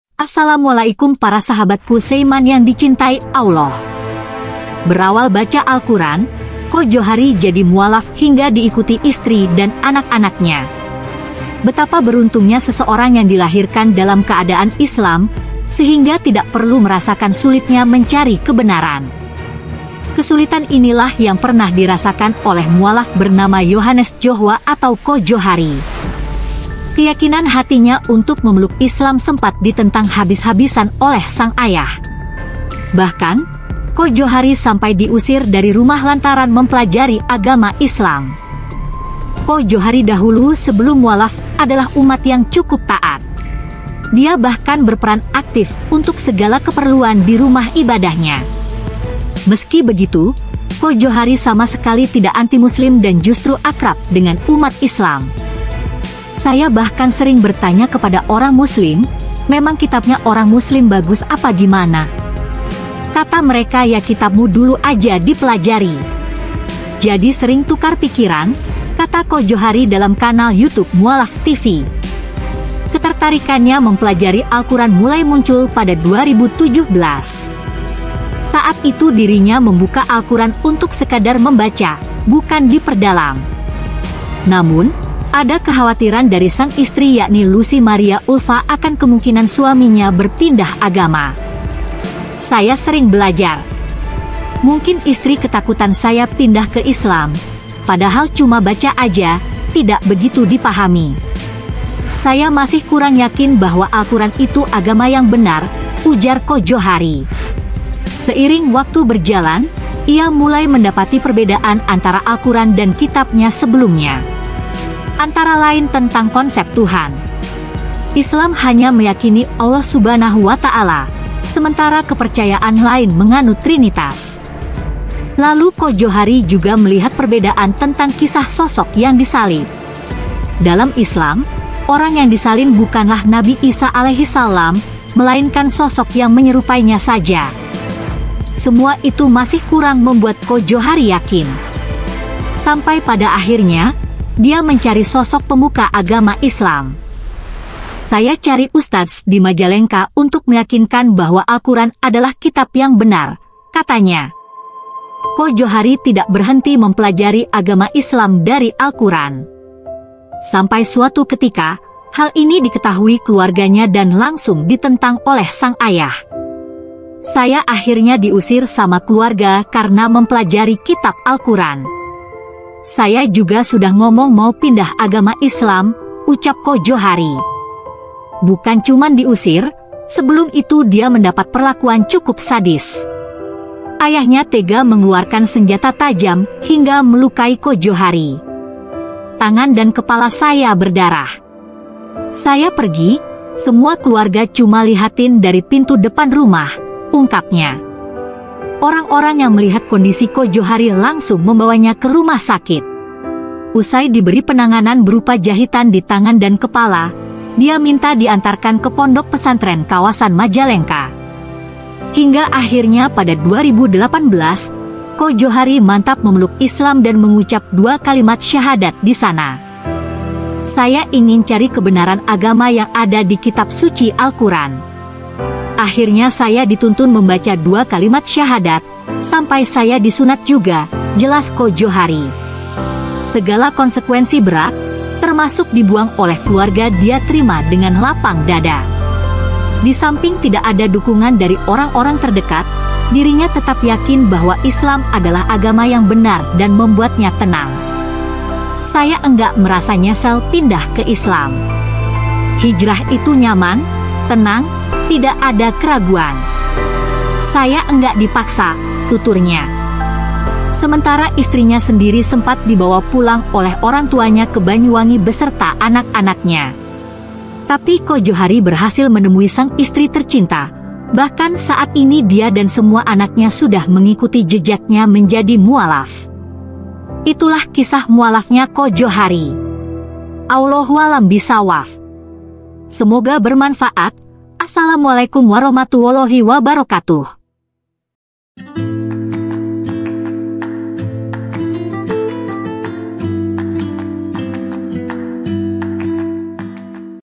Kisah Mualaf: Yakin Islam Hingga Diusir dan Hampir Dibunuh Oleh Keluarganya
Pada video ini menceritakan Kisah Mualaf: Yakin Islam Hingga Diusir dan Hampir Dibunuh Oleh Keluarganya. Inilah kisahnya!